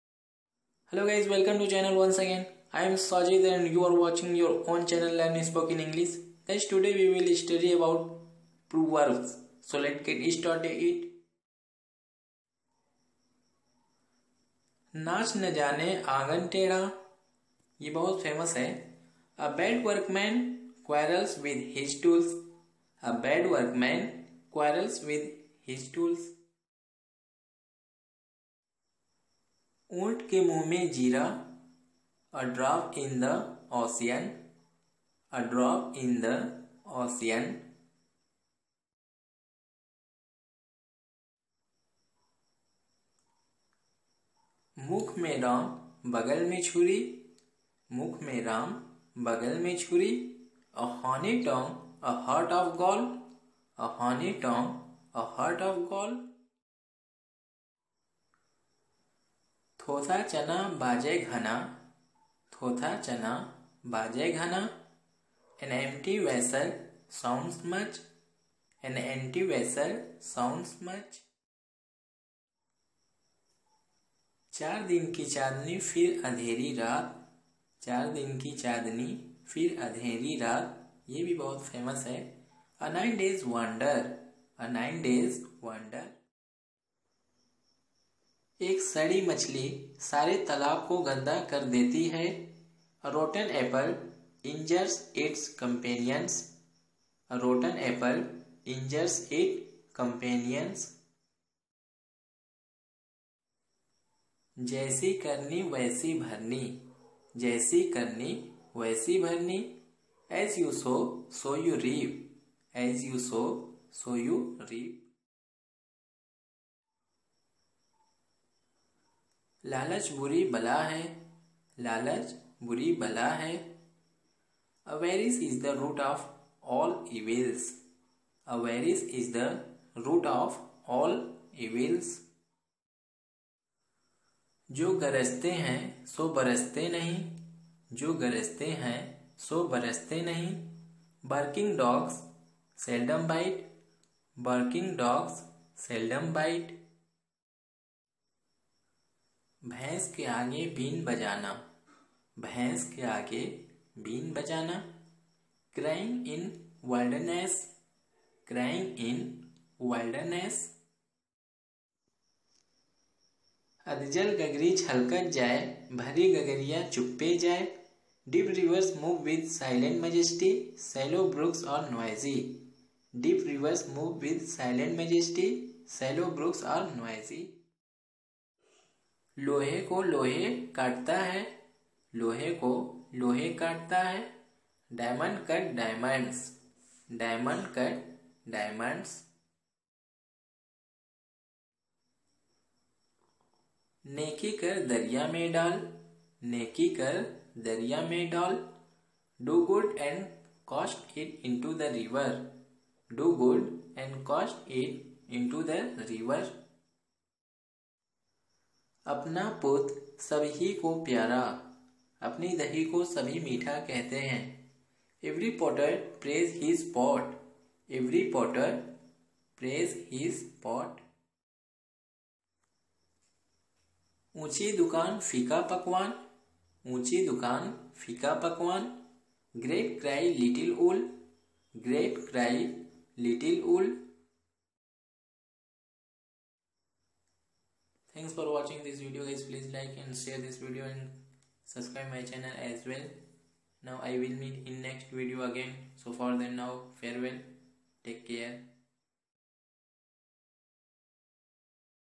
Important Proverbs।महत्वपूर्ण कहावतें।Spoken English।Latest topic।Full video।Class।By Sazid Sir
Hello everyone, We have given in this video"Important Some proverbs"which these all Proverbs are very common who everyone speaks daily use language.  Must watch it and make your knowledge strong.